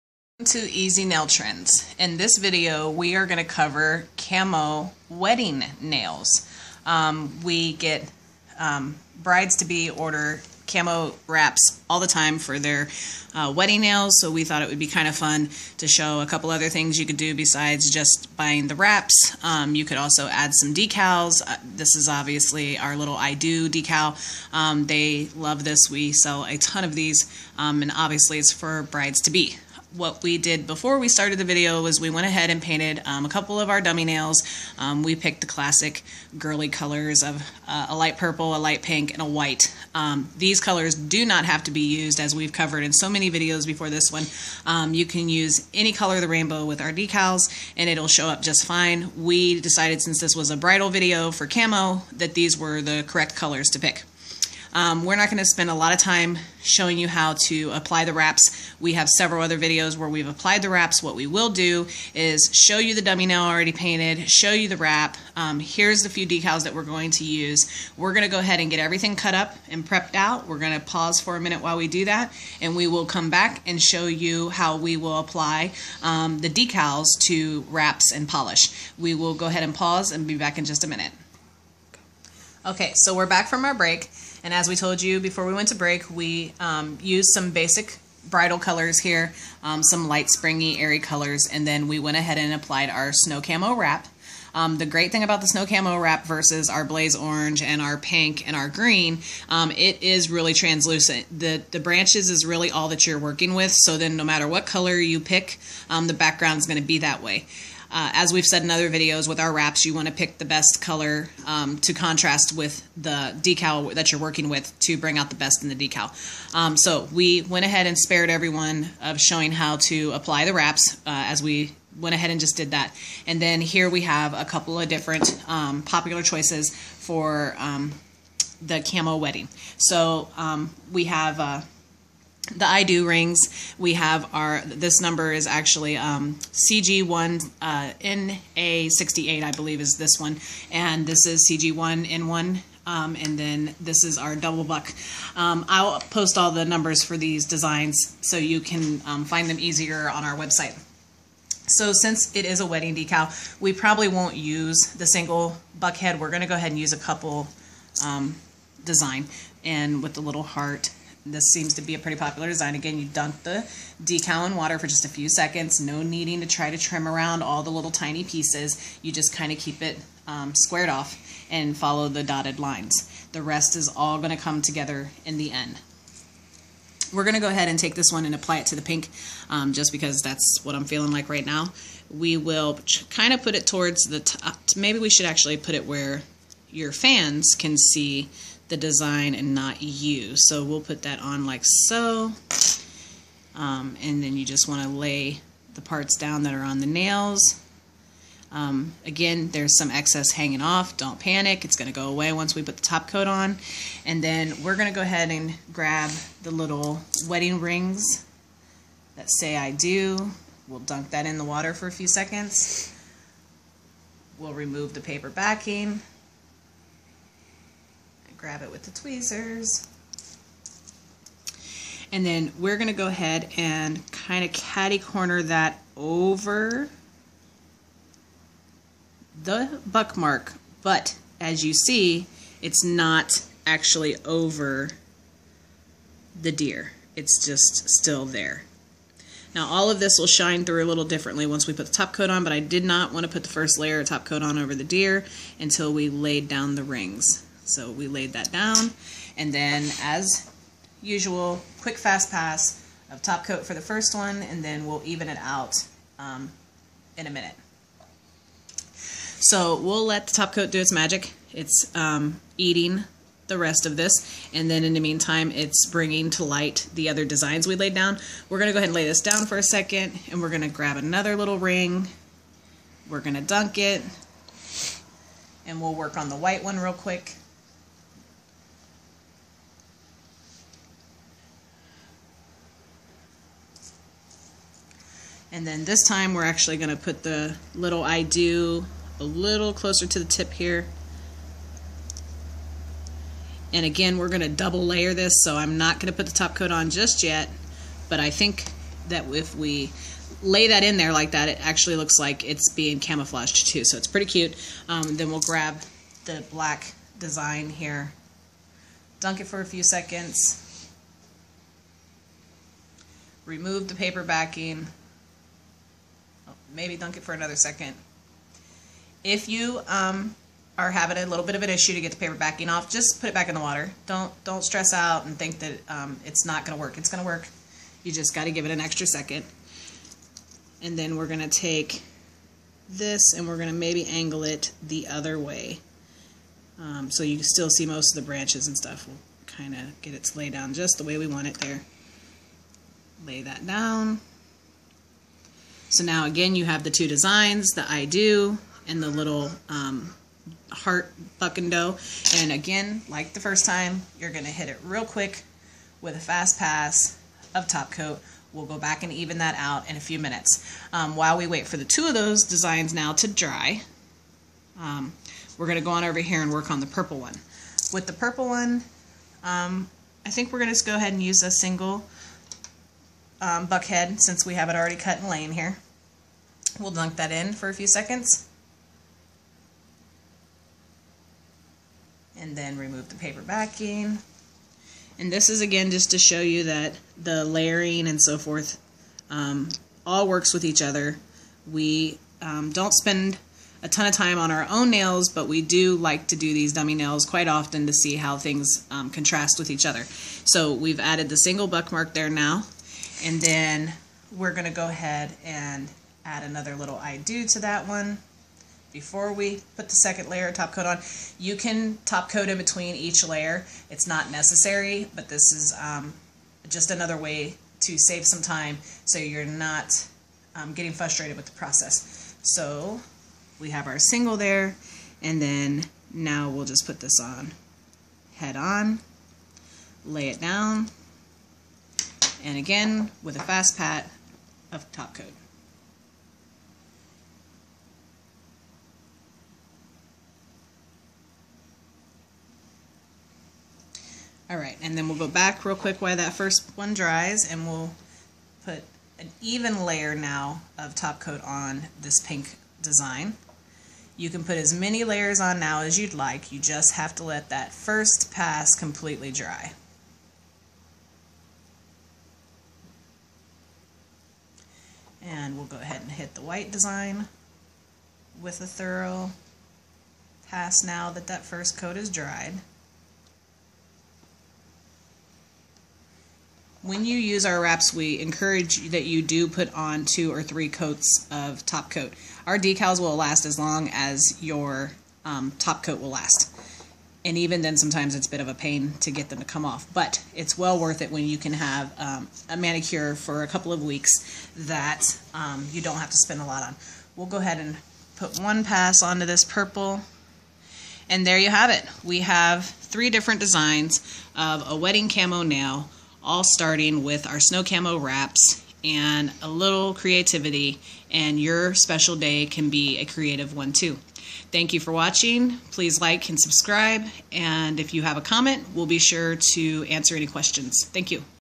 Easy White Camo Wedding Nails
Camo Wedding Nails , camoweddingnails How To Create Your Dream Country Camo Nails . Item # As Follows cg1na1 , cg1na68 , cg1712na , cg712na and Snow Camo Wraps . You can fine these items on our website easynailtrends.com